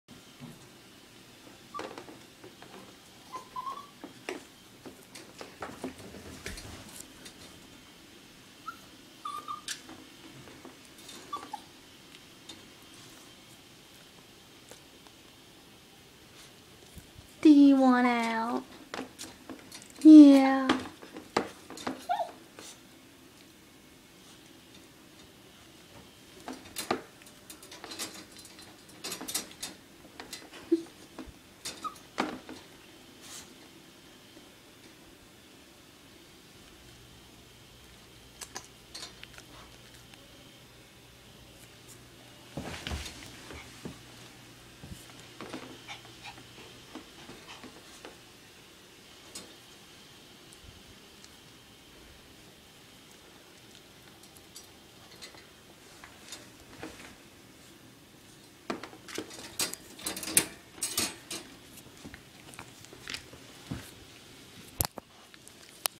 Yorkie Wants Out Of His Crate!
Mikey hates being locked up in his crate when we're gone somewhere.